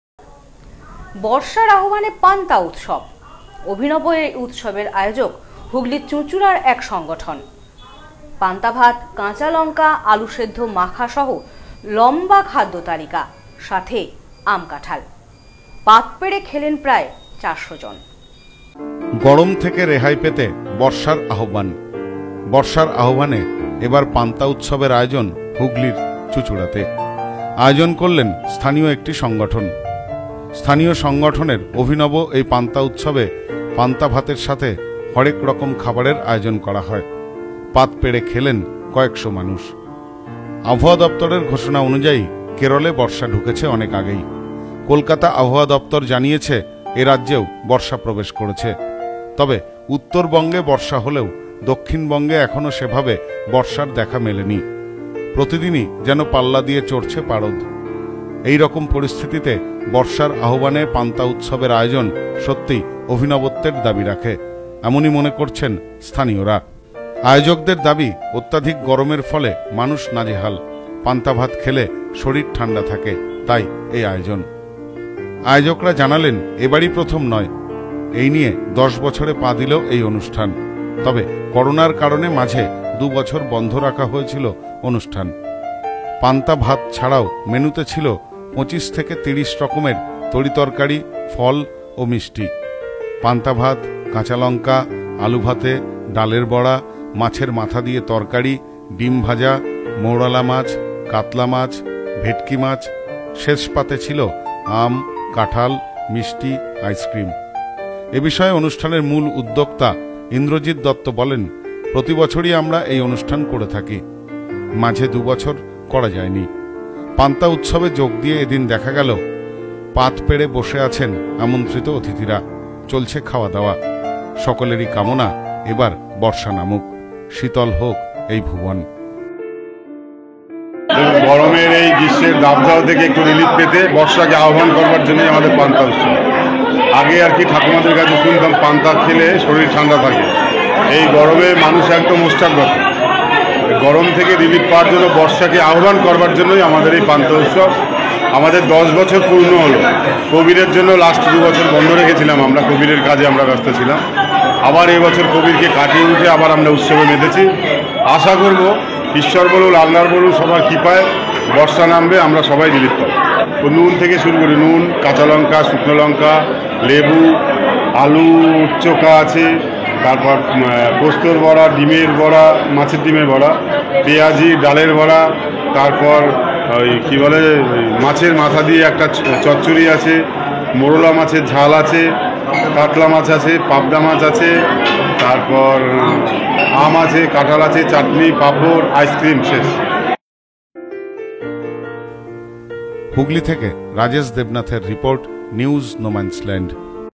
বর্ষার আহ্বানে পান্তা উৎসব। অভিনব এই আয়োজন হুগলীর চুঁচুড়ার এক সংগঠনের।
বর্ষার আহ্বানে পান্তা উৎসব। অভিনব এই আয়োজন হুগলীর চুঁচুড়ার এক সংগঠনের। পান্তাভাত, কাঁচালঙ্কা, আলু সেদ্ধ মাথা সহ লম্বা খাদ্য তালিকা। সাথে আম, কাঁঠাল। দুপুরে পাত পেড়ে খেলেন প্রায় ৪০০ জন।